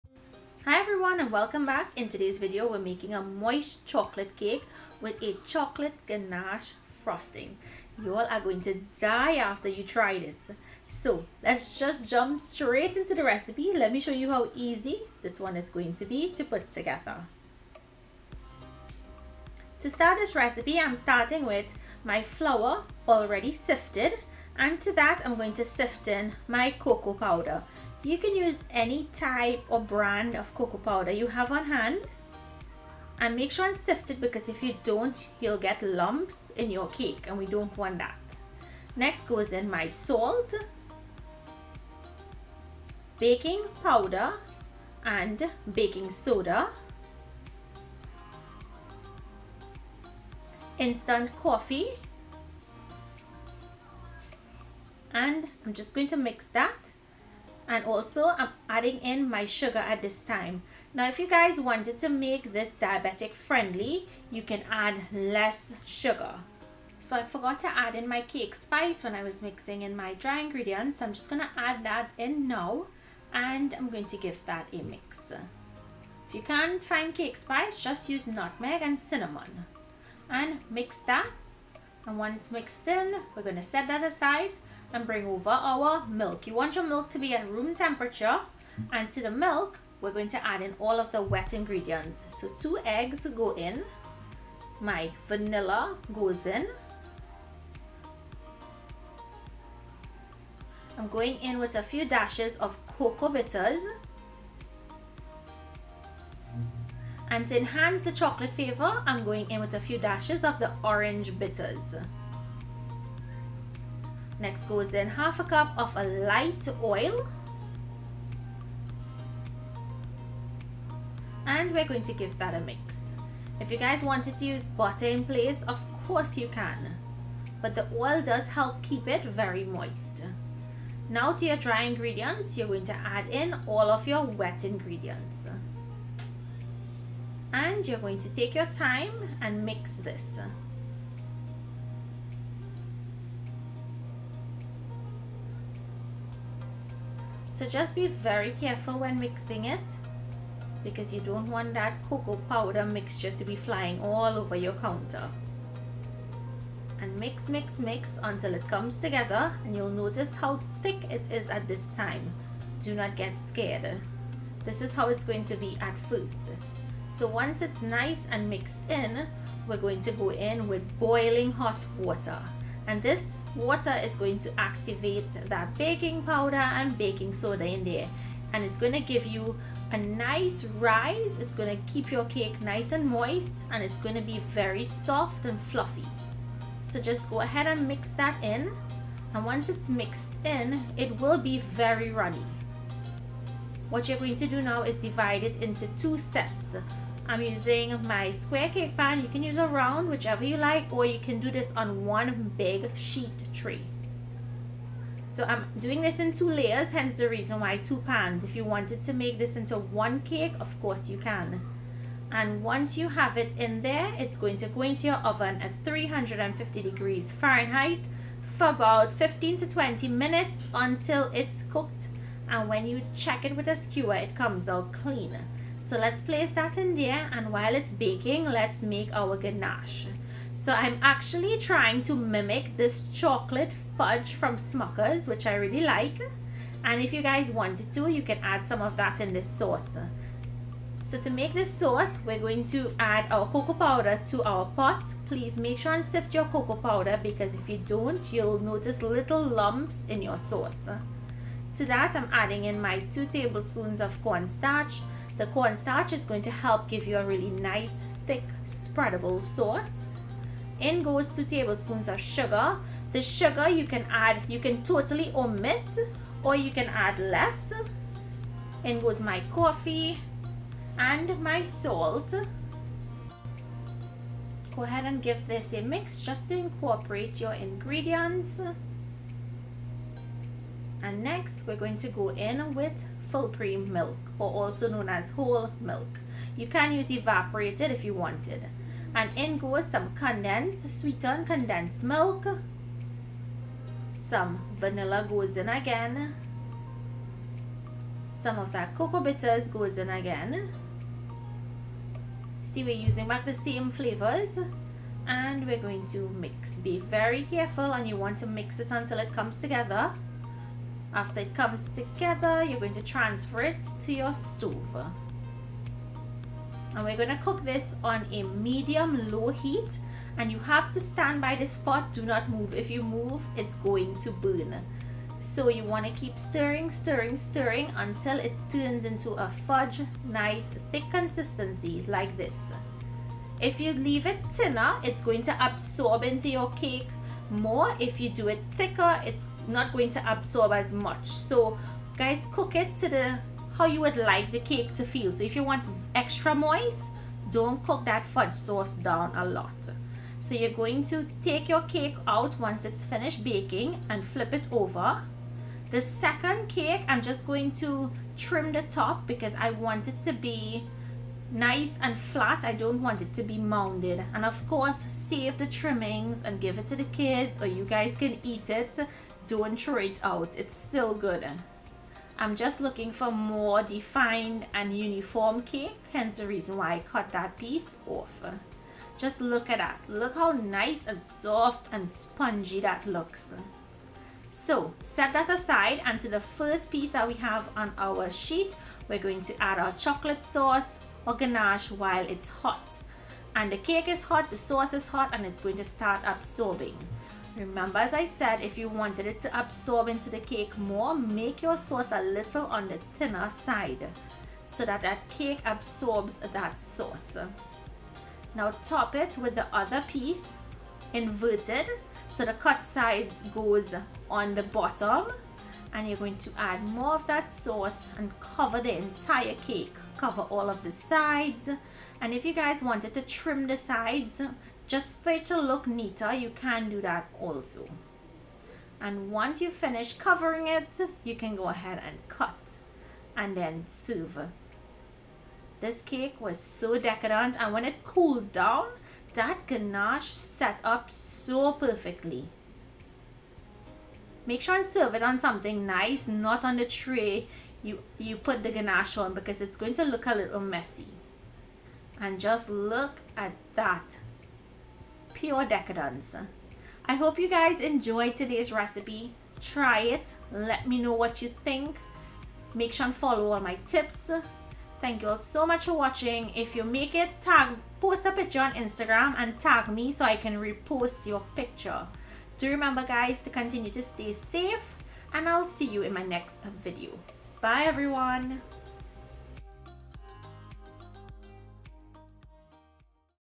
Moist Chocolate Cake with Chocolate Ganache Fudge Topping - Episode 2029
2 cups all purpose flour
3/4 cups cocoa powder
1/4 tsp salt
1 1/2 tsp baking powder
1 1/2 tsp baking soda
2 tsp instant coffee
1 1/4 cups sugar
1/2 tsp cake spice
1 cup full cream milk at room temperature
2 eggs
1 tsp vanilla
8 dashes Angostura Cocoa bitters
4 dashes orange bitters
1/2 cup light oil - vegetable,canola,soyabean,soybean, olive oil
1 cup boiling water

3 heaping tbsp cocoa powder sifted
2 tbsp corn starch
2 tbsp sugar
2 tsp instant coffee
1/4 tsp salt
3/4 cup full cream milk
2 tbsp condensed milk
1 tsp vanilla
3 dashes cocoa bitters

My Silicone Spoons
My Fireside pot
Mini silicone spoons
Danish Dough Hook